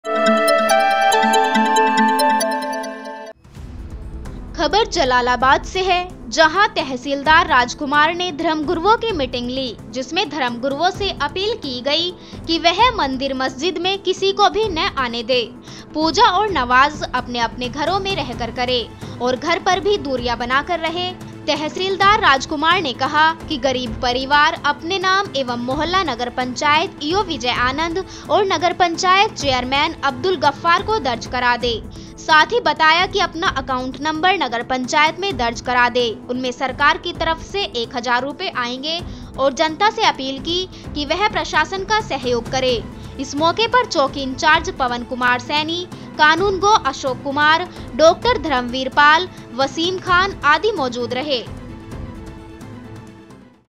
शामलीः तहसीलदार ने धर्मगुरूओं के साथ की मीटिंग by samachar today
खबर जलालाबाद से है। जहां तहसीलदार राजकुमार ने धर्मगुरुओं की मीटिंग ली जिसमें धर्मगुरुओं से अपील की गई कि वह मंदिर मस्जिद में किसी को ना आने दे। पूजा और नवाज अपने-अपने घरों में रह कर करें। और घर पर भी दूरियां बनाकर रहे। तहसीलदार राजकुमार ने कहा कि गरीब परिवार अपने नाम एवं मोहल्ला नगर पंचायत ई ओ विजय आनंद और नगर पंचायत चेयरमैन अब्दुल गफ्फार को दर्ज करा दें साथ ही बताया अपना अकाउंट नंबर नगर पंचायत में दर्ज करा दे उनमें सरकार की तरफ से ₹1000 आएंगे। और जनता से अपील की वह प्रशासन का सहयोग करें। इस मौके पर चौकी इंचार्ज पवन कुमार सैनी, कानूगो अशोक कुमार, डॉ धर्मवीर पाल, वसीम खान आदि मौजूद रहे।